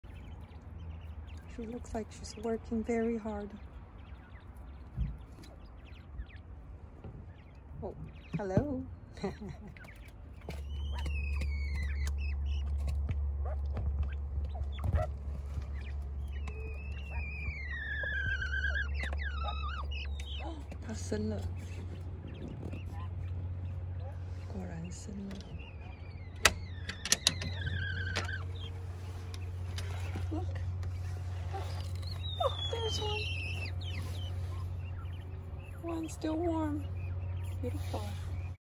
Quail cries before laying
This is what the girls sound like when they are laying...some of them could cry for 20-30 minutes!